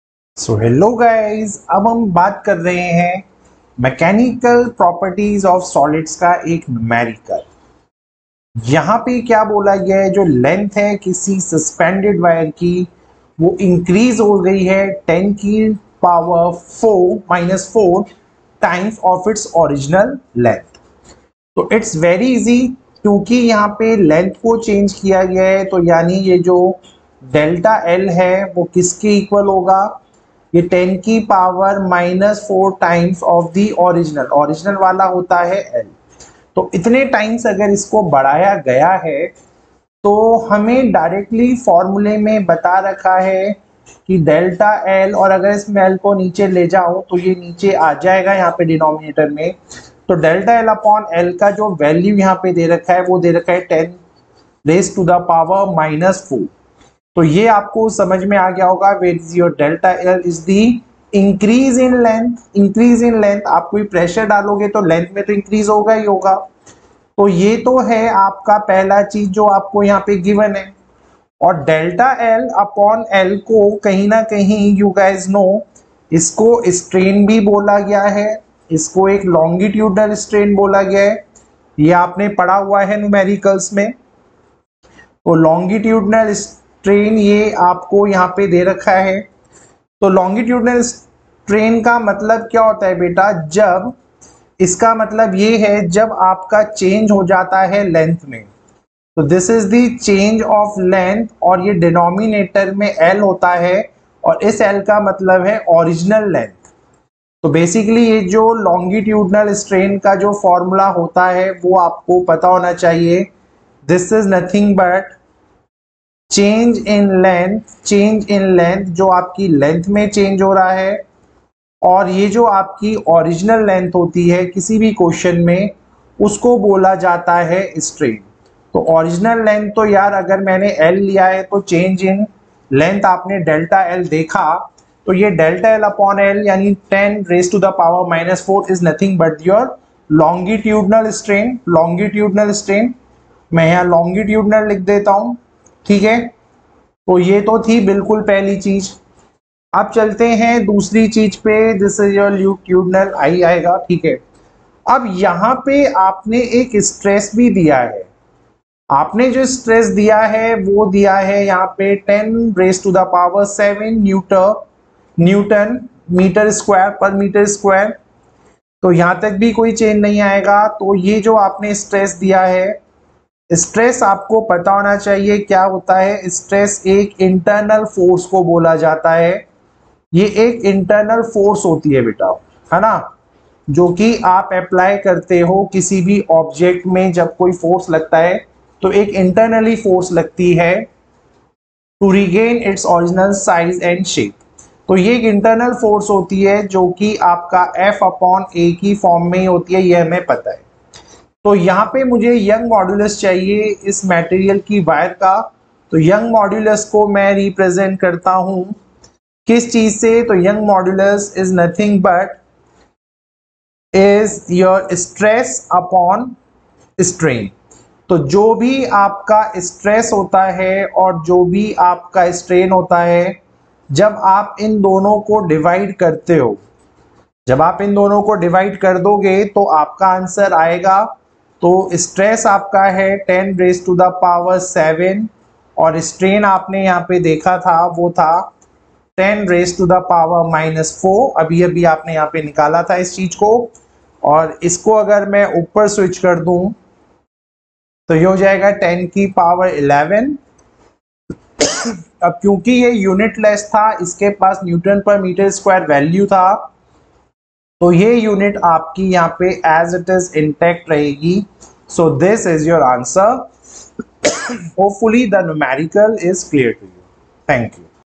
Ques 1: MECHANICAL PROPERTIES OF SOLIDS || Numerical of SL ARORA
The length of a suspended wire increases by 10 ^−4 of its original length when a stress of 10^7Nm−2 is applied on it. Calculate the Young's modulus of the material of the wire.